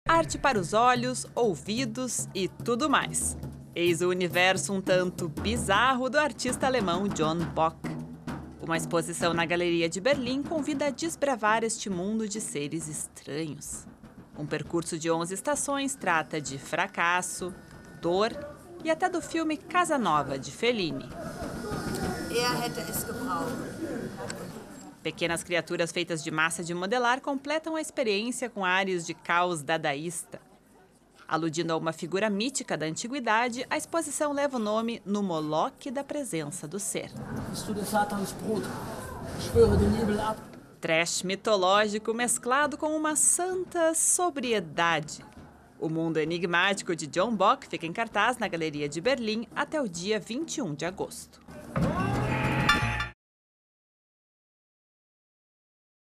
O mundo enigmático do artista alemão John Bock
Exposição na Galeria de Berlim convida o visitante a desbravar o mundo de seres estranhos criados pelo artista alemão.